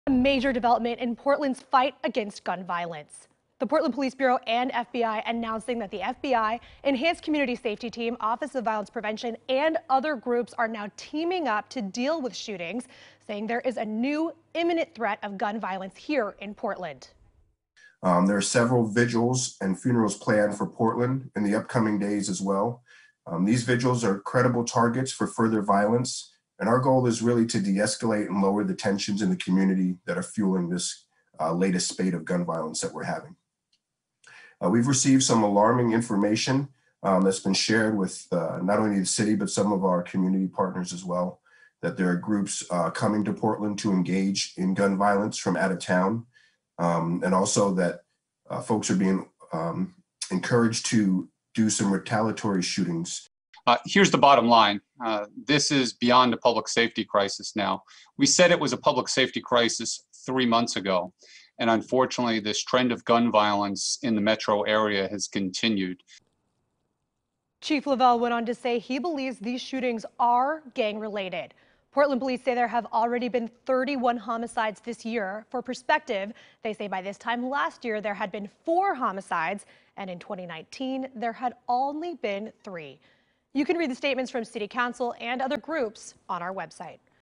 FBI: Portland gun violence 'beyond a public safety crisis'
With news of an "imminent" threat of gun violence in Portland, Police Chief Chuck Lovell and an FBI official spoke about violence reduction efforts during a media briefing on Saturday.